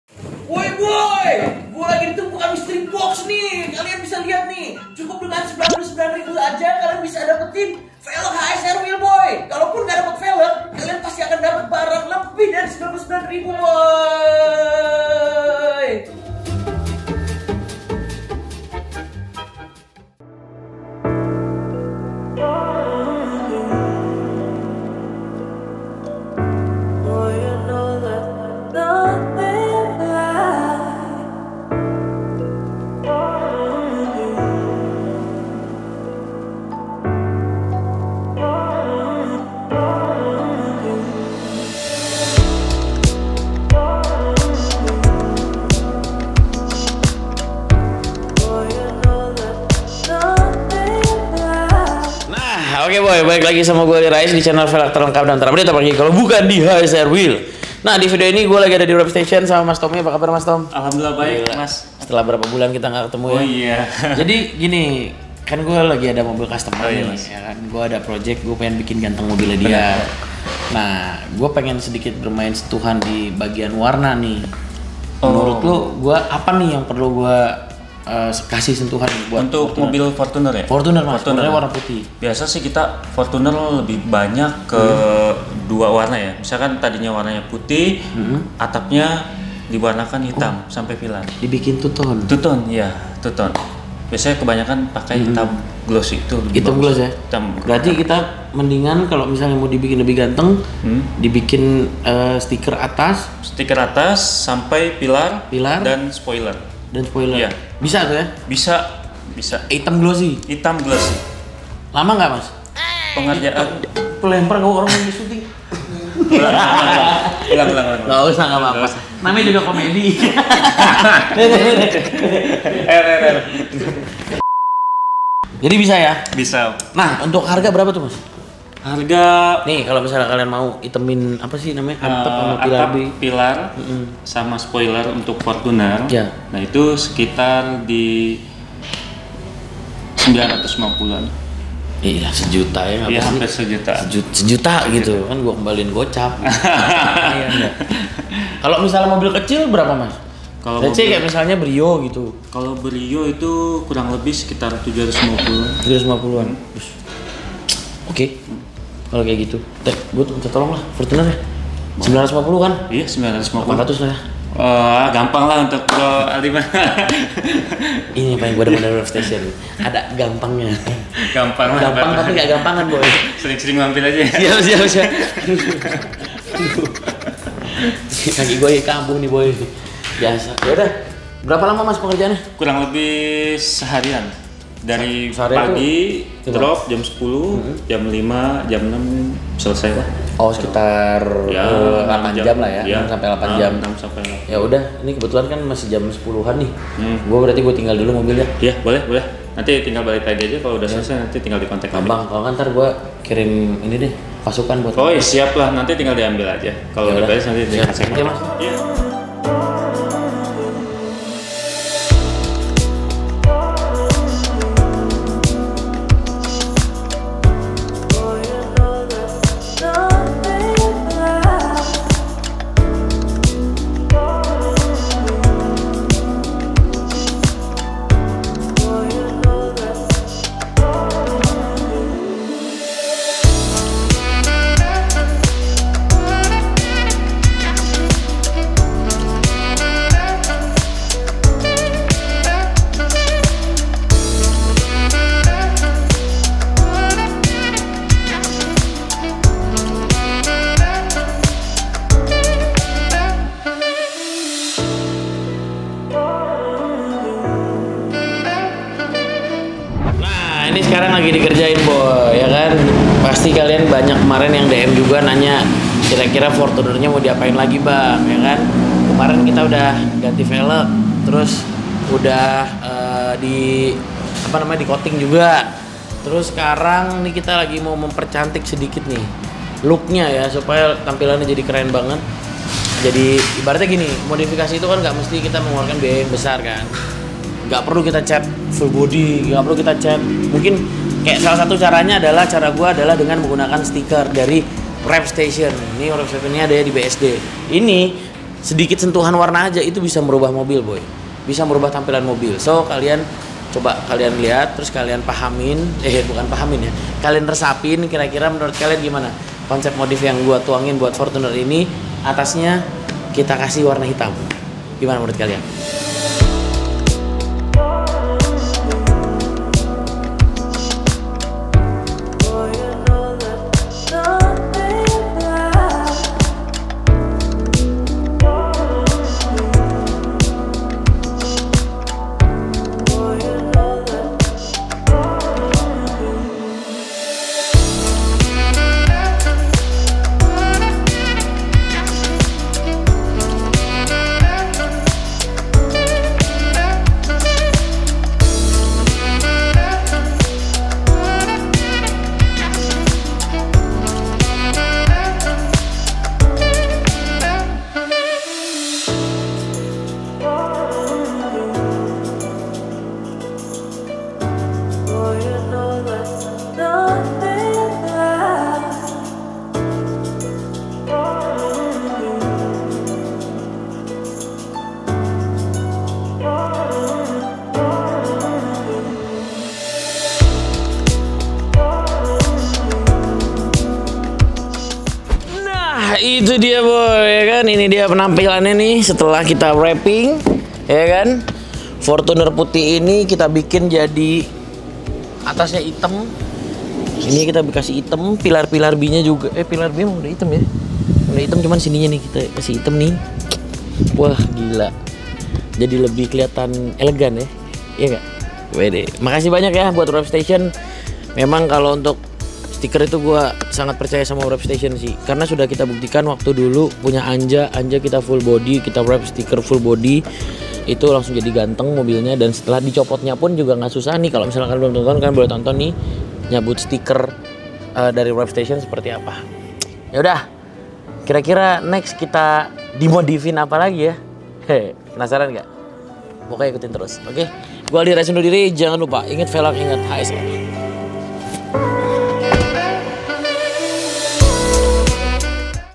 Baru Keluar Dari Dealer, Langsung Di Bungkus !
Kali ini team HSR berangkat buat modifikasi Mobil Fortuner yang baru aja keluar dari dealer. Langsung kita wrapping boy !